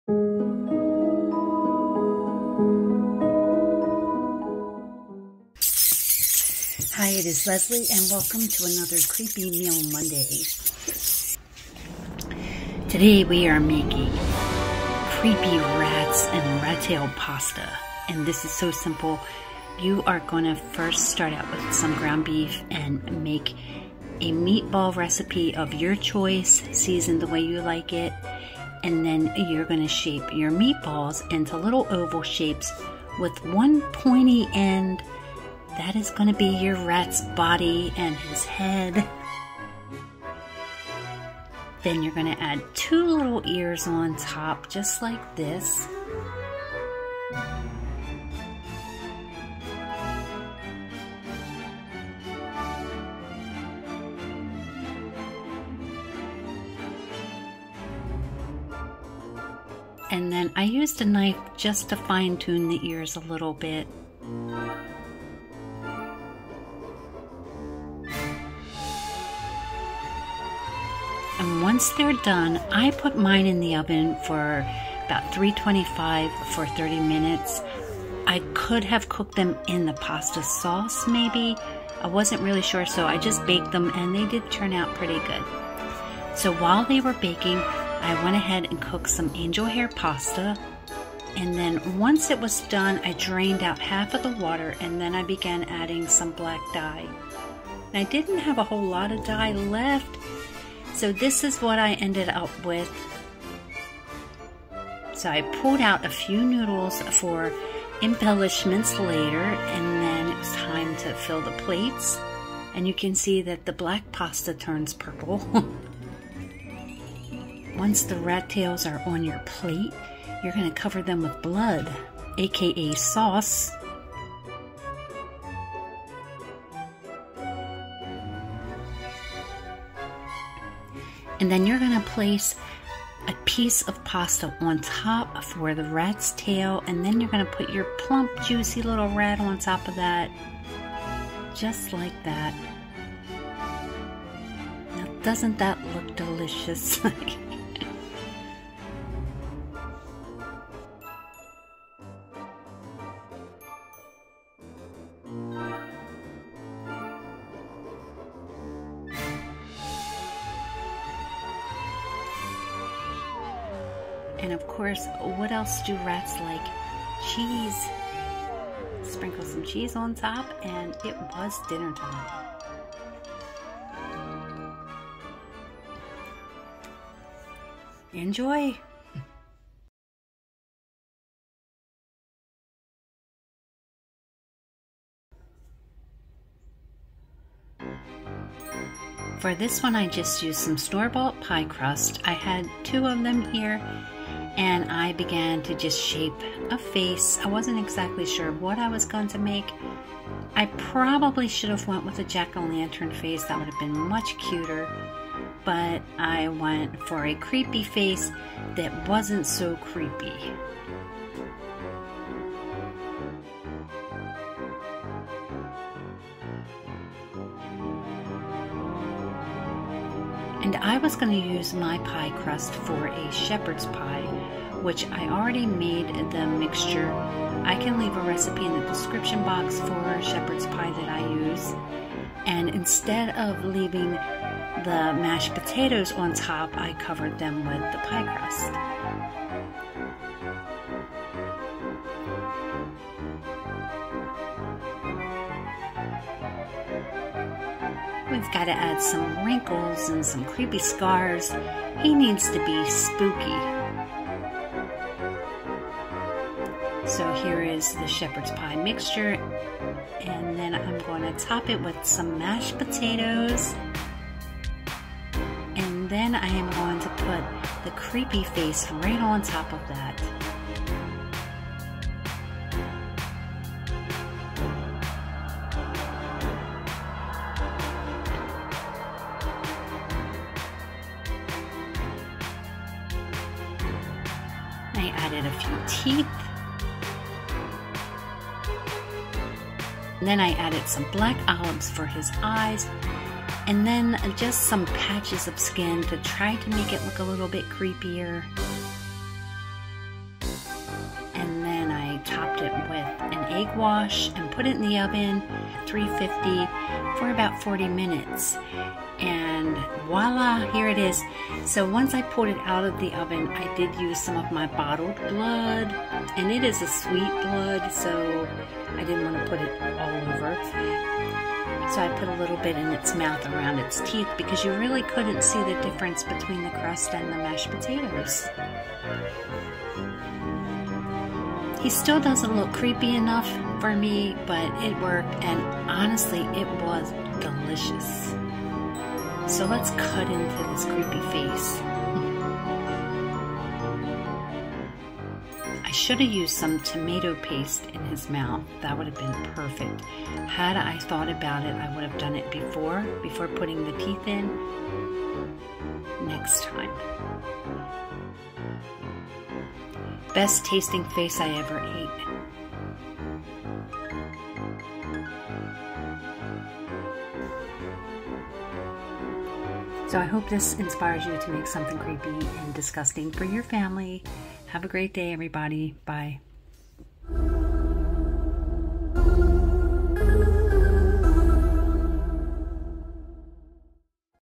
Creepy Meal Monday #3 ~ Rat Tail Pasta and a Creepy Face Pie
Hi Friends. It's another creepy meal Monday. I've been having fun with these disgusting concoctions! Today's video features a yummy and very simple Rat Tail Pasta and a creepy face on a Shepard's pie. I added an easy Shepard's pie recipe below. For mine I used a bottom and top layer of pie crust and instead of Ketchup, I added a can of cream of celery soup - which was delicious and very hardy. I hope you enjoy today's fun!!

Shepard's Pie

Ingredients
1 pound ground beef

⅓ cup ketchup

1 teaspoon Worcestershire sauce

8 ounces frozen mixed vegetables (such as carrots, peas, and corn), thawed

¼ cup (1 ounce) shredded Cheddar (optional)

1 16-ounce package mashed potatoes, refrigerated or frozen and thawed

Directions
Heat oven to 400°F.

Place the beef in a large skillet and cook over medium-high heat until no trace of pink remains, about 5 minutes.

Spoon off and discard any fat. Stir in the ketchup and Worcestershire. Add the vegetables and cook, stirring, for 1 minute. Spoon into a baking dish.

Mix the cheese (if desired) with the potatoes in a bowl. Spread over the beef and bake until heated through, 10 minutes. Divide among plates.

Easy Shepherd's Pie